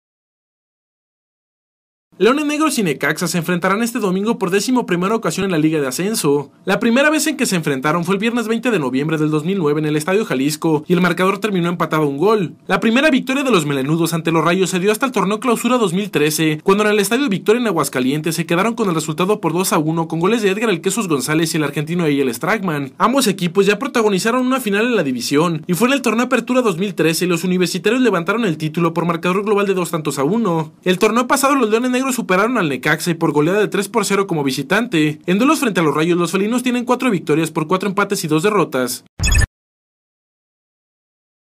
Señal Deportiva: En duelos ante el Necaxa, los Leones Negros tienen saldo a favor
La primera vez en que se enfrentaron fue el   viernes 20 de noviembre  del 2009 en el estadio Jalisco y el marcador termino empatado a 1 gol.